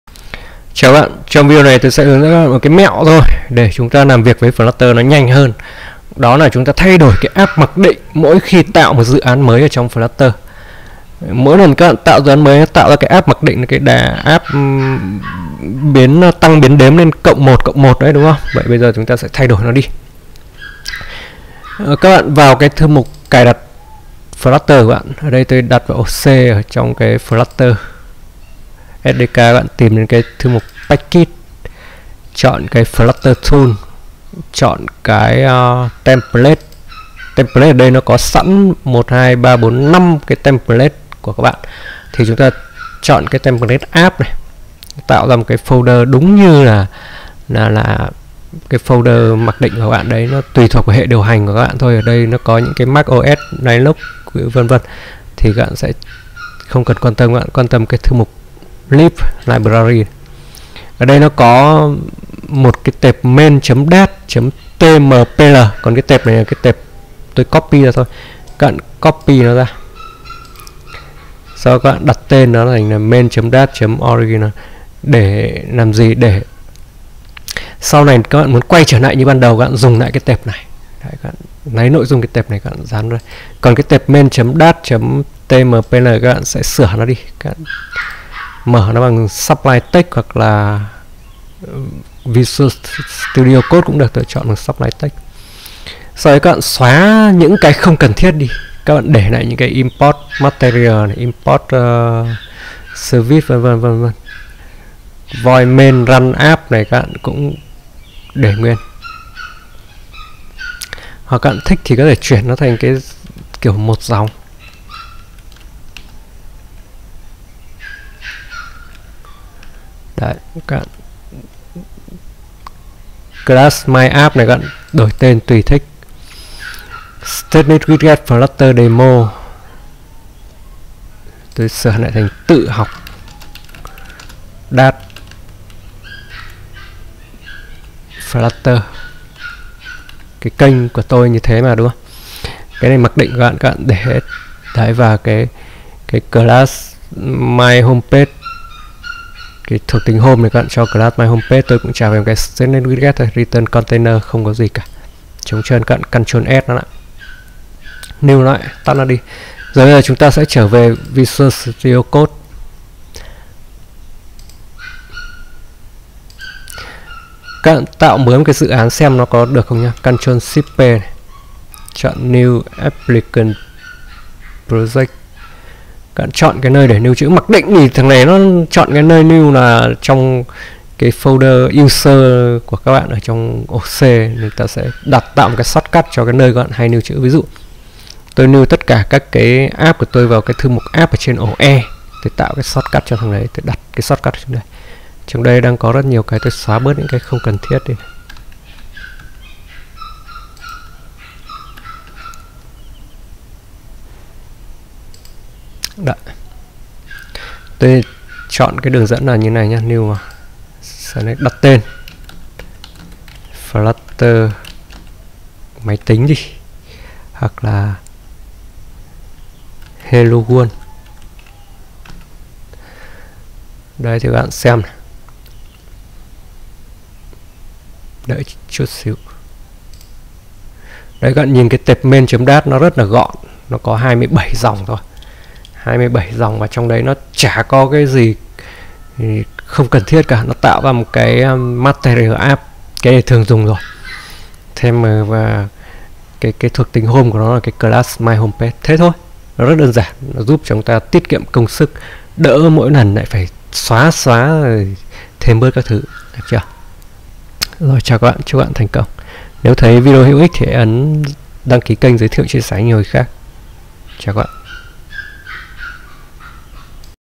1.5 Hướng dẫn cách thay đổi app mẫu mặc định của Flutter
✅Hướng dẫn cách thay đổi app mẫu (template Counter App) mặc định của Flutter mỗi khi tạo dự án mới
✅Các bạn sửa file ở thư mục './{your-flutter-install}/packages/flutter-tools/templates/app/'